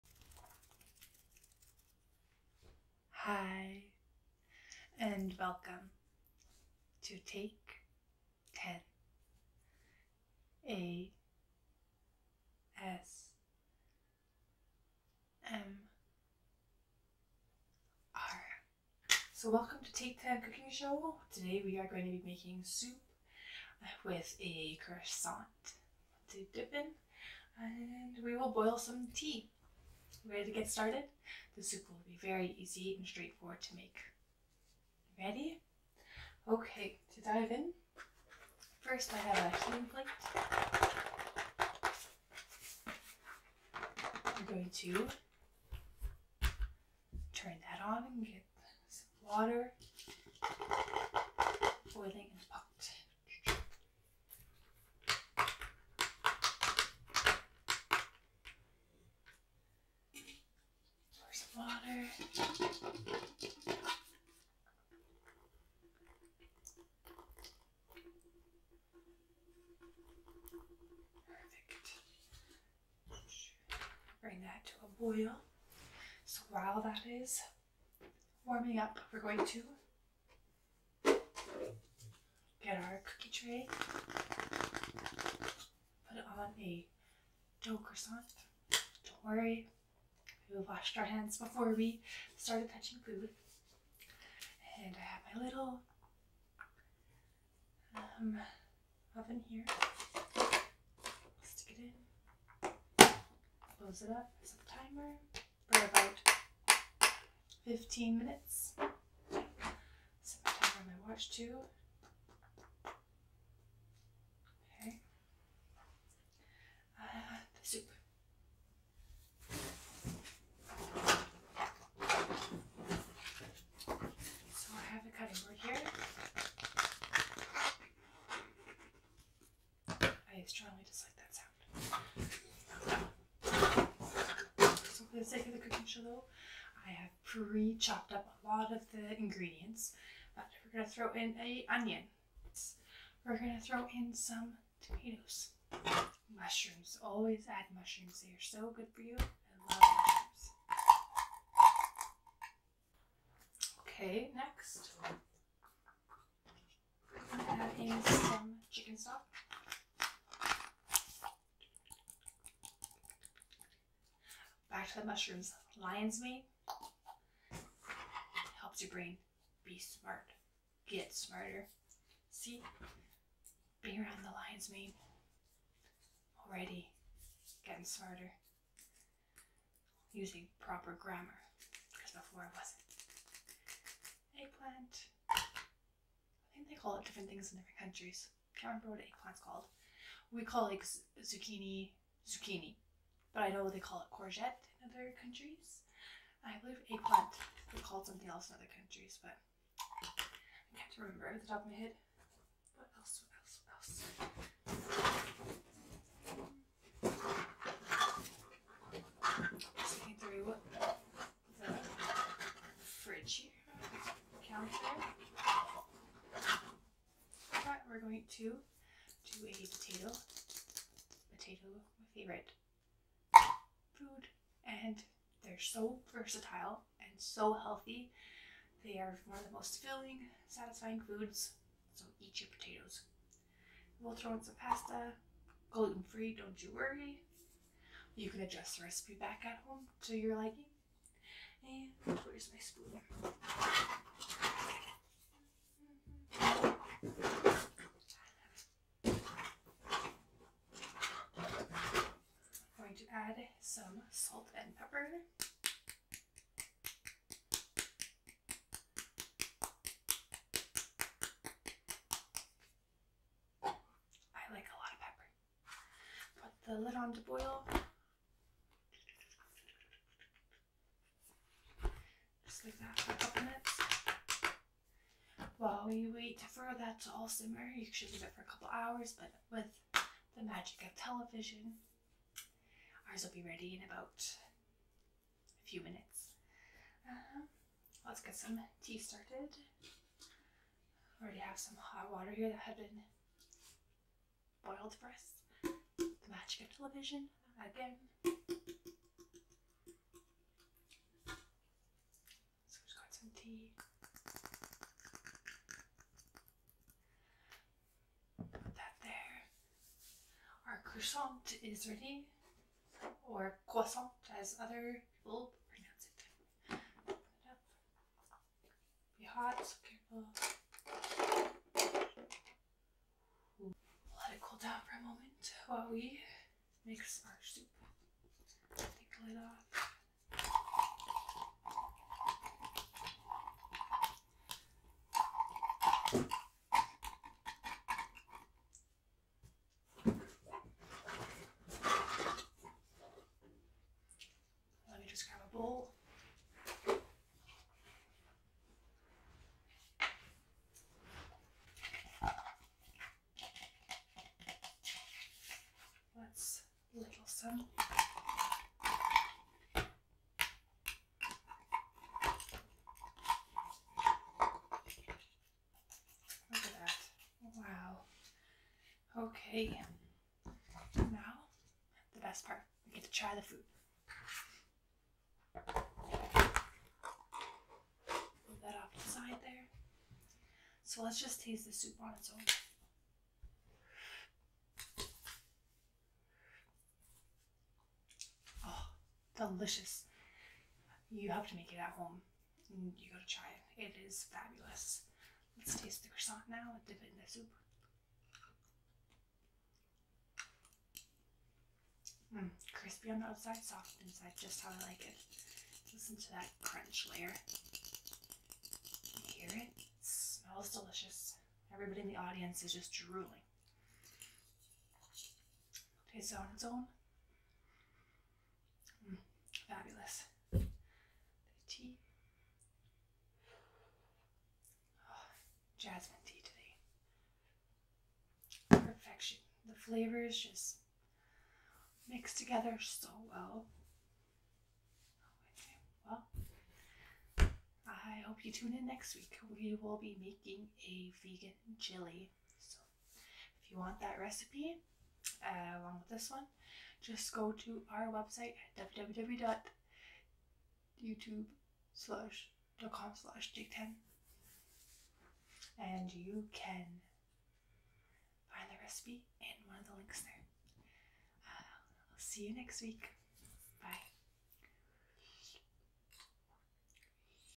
Whispering Whisks: ASMR Fake Cooking Show with Play Food Soup
Welcome to "Whispering Whisks," an ASMR video that takes you on a delightful journey through a whimsical fake cooking show. In this playful and relaxing experience, I will be your host as we create a delicious imaginary soup using plastic play food. Using my normal voice, I'll guide you through the steps of this imaginative culinary adventure. As we mix and stir, the gentle clinks and rustles of the play food will create a soothing symphony of sound. The pretend ingredients will come to life in your mind's eye as you imagine the aroma and flavors of our fantastical soup. The soft whispers of instructions and descriptions will transport you into a world where cooking is a serene and calming activity. Sit back, relax, and let your imagination run wild as we create a masterpiece with our plastic play food. Whether you're a cooking enthusiast or simply seeking a moment of whimsy and relaxation, this ASMR fake cooking show will provide a delightful escape from the ordinary. Immerse yourself in the comforting ambiance of "Whispering Whisks" and let the gentle sounds and visuals whisk you away to a world of culinary imagination and tranquility.